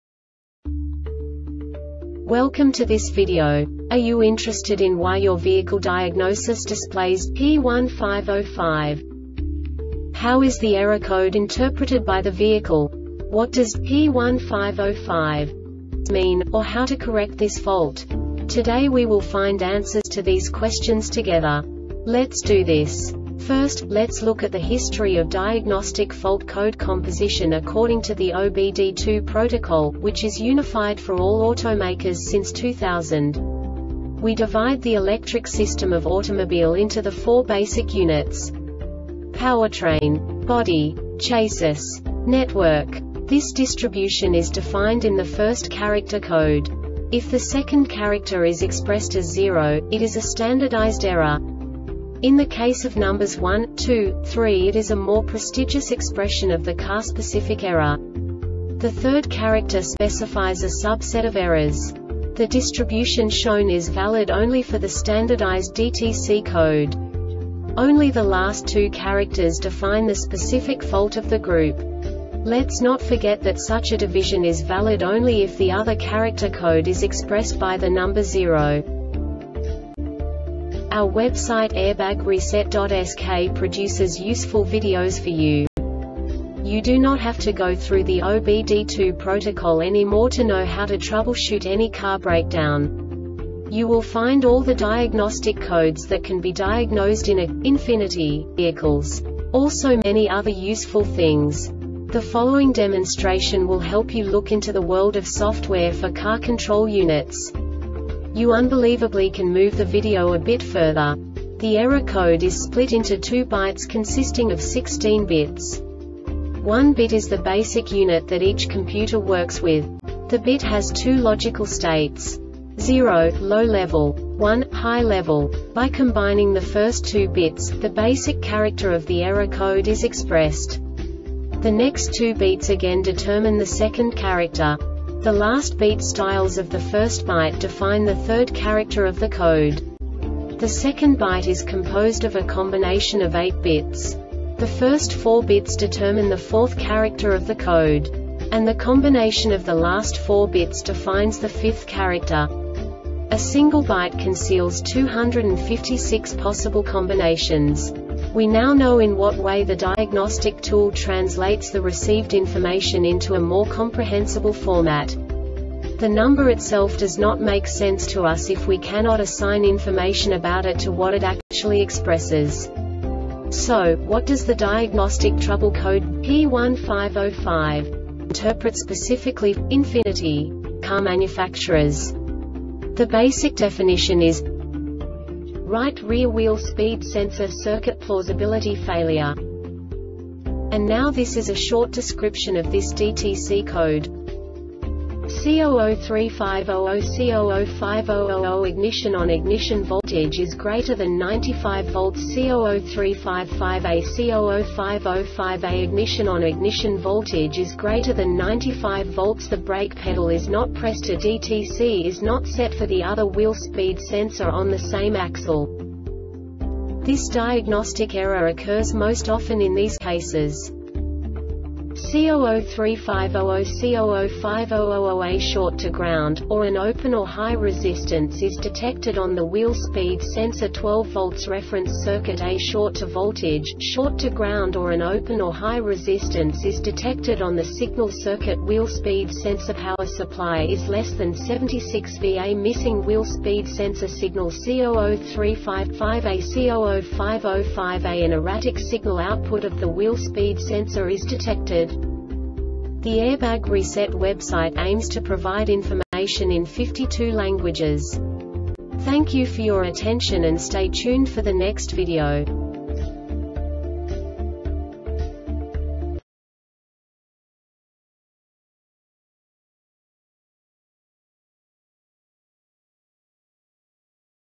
DTC Ford P1505 Short Explanation
Contents:
0:21 Basic DTC analysis according to OBD2 protocol standard.
1:48 Insight into programming
2:58 A diagnostically understandable description of the Diagnostic Trouble Code
3:05 Basic error definition

Make:
Ford
Code:
P1505
Definition:
Right Rear Wheel Speed Sensor Circuit Plausibility Failure
Description:
C0035 00-C0050 00 Ignition ON.Ignition voltage is greater than 9.5 V.C0035 5A-C0050 5A Ignition ON.Ignition voltage is greater than 9.5 V.The brake pedal is not pressed.A DTC is not set for the other wheel speed sensor on the same axle.
Cause:
C0035 00-C0050 00A short to ground,or an open or high resistance is detected on the wheel speed sensor 12 V reference circuit.A short to voltage,short to ground or an open or high resistance is detected on the signal circuit.Wheel speed sensor power supply is less than 7.6 V.A missing wheel speed sensor signal.C0035 5A-C0050 5A An erratic signal output of the wheel speed sensor is detected.